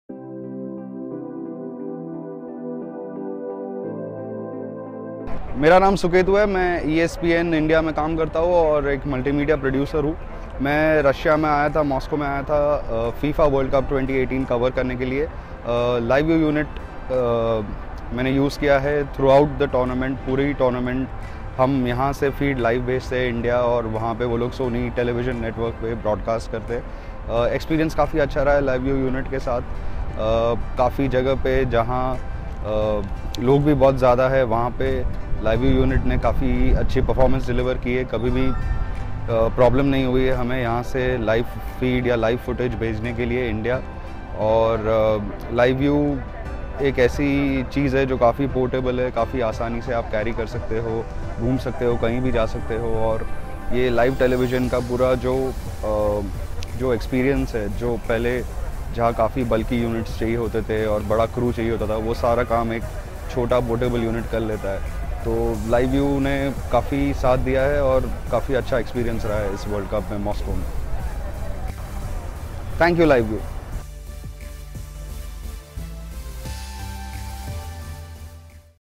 ESPN India Using LiveU at the 2018 World Cup
Suketu Mehta, Multimedia Producer at ESPN India speaks about his experience using LiveU during the 2018 FIFA World Cup™